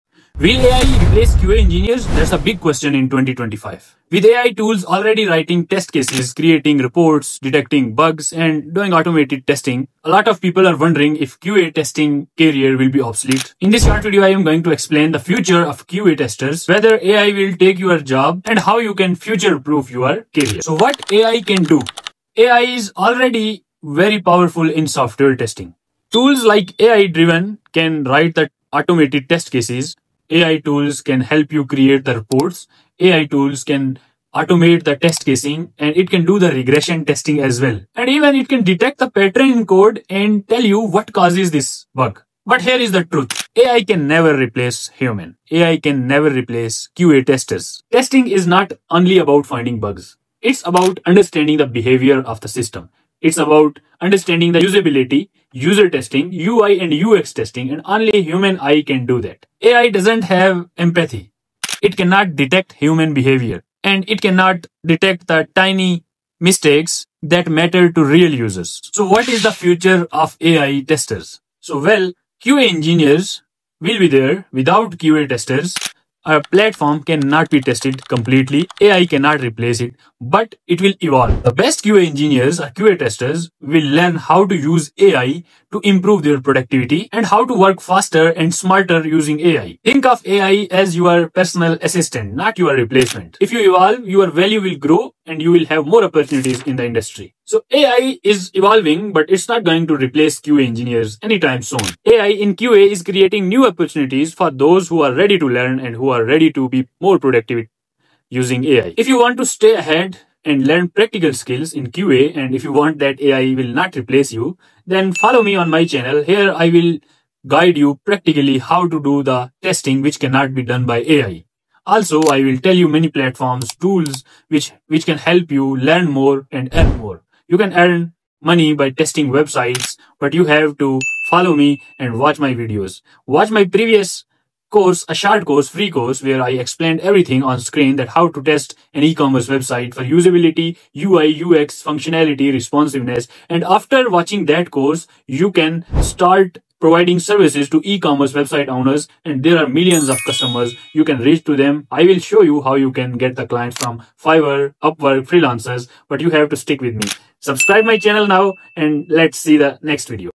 Will AI Replace QA Engineers in 2025? The Truth About QA Jobs
Will AI Replace QA Engineers? The Honest Truth About Your QA Career in 2025
The biggest question every QA engineer is asking: Will AI take my testing job? With AI tools already writing test cases, detecting bugs, and automating reports, many software testers are worried about their future. In this video, I reveal the REAL impact of AI on QA careers and show you exactly how to future-proof your testing skills.
What You'll Discover:
✅ What AI can and CANNOT do in software testing
✅ Why QA engineers will NEVER be fully replaced
✅ How to use AI tools to boost your testing career
✅ Future-proof QA skills that AI cannot replicate
✅ The evolution of manual testing in the AI era

Key Insights:
• AI excels at repetitive tasks but lacks human judgment and creativity
• Exploratory testing, usability testing, and user experience evaluation still require humans
• Smart QA engineers will learn to work WITH AI, not compete against it
• Manual testing skills remain highly valuable and AI-resistant

🎯 Perfect for:
• QA engineers concerned about job security
• Software testers wanting to stay relevant
• Beginners starting their QA career
• Anyone curious about AI's impact on testing jobs

👍 LIKE if this eased your AI concerns!
💬 COMMENT: Are you worried about AI replacing your QA job?